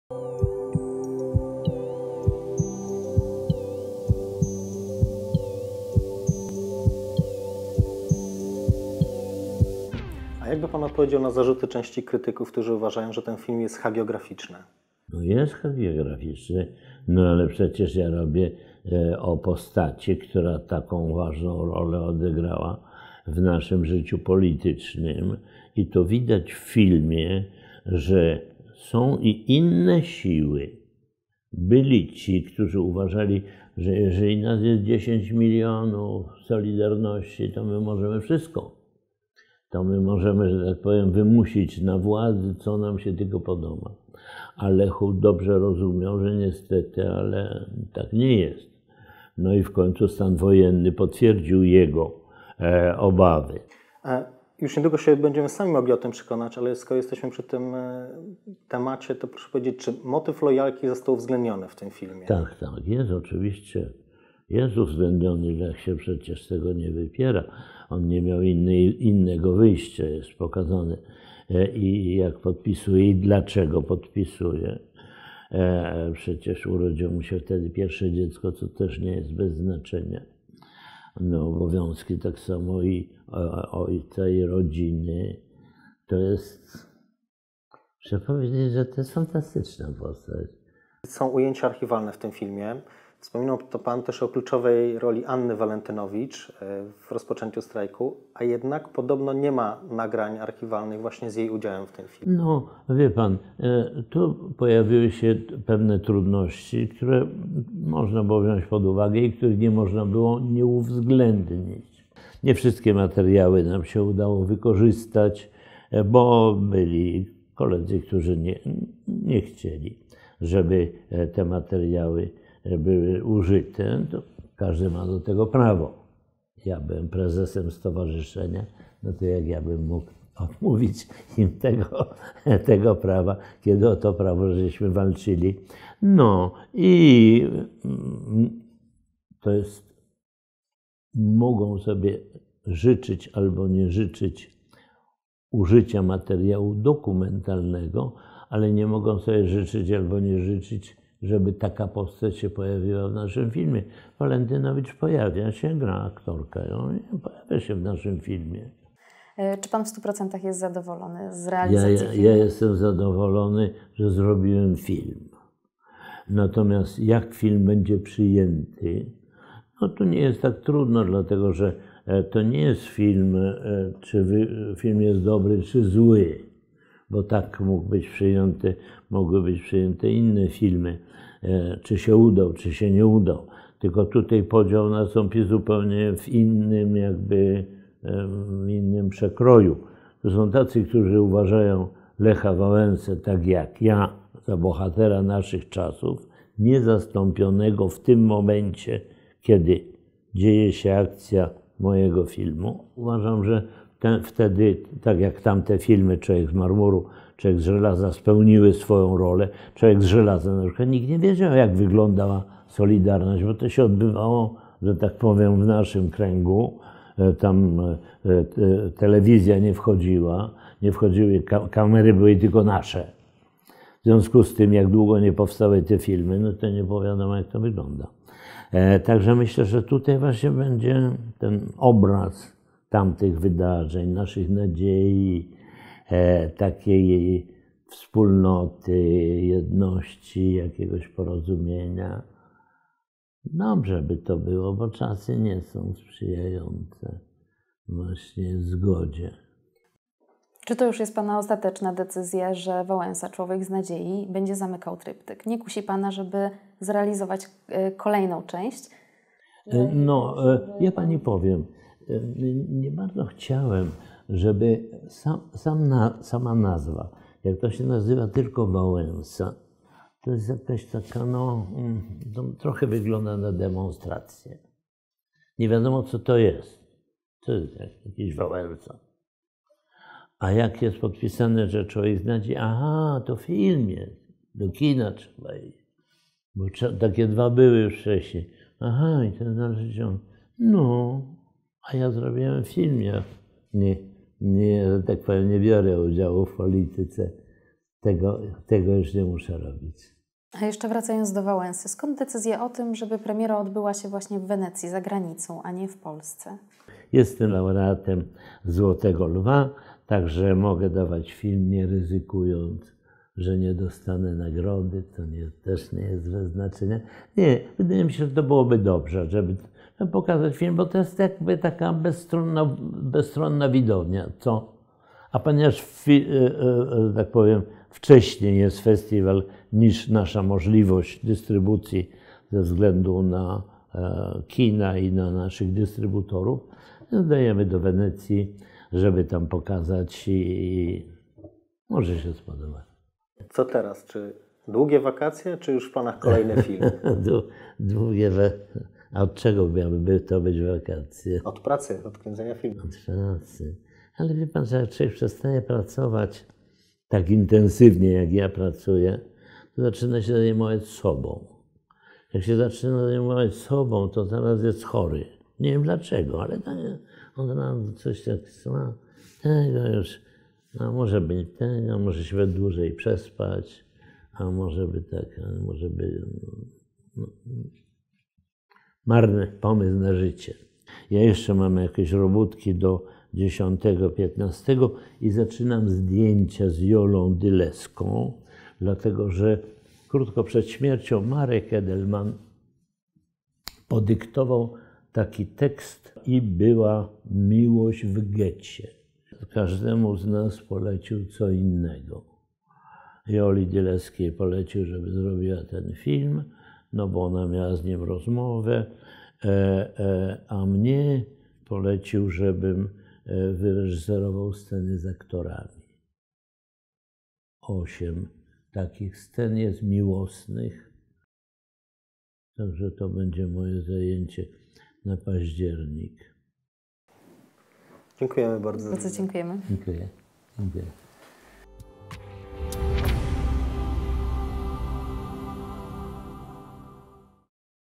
Wywiad z Andrzejem Wajdą - część 2
Zapraszamy do obejrzenia drugiej części wywiadu z Andrzejem Wajdą - reżyserem filmu: "Wałęsa. Człowiek z nadziei". Film powstał we współpracy z Orange, a na ekranach kin będzie już 4 października.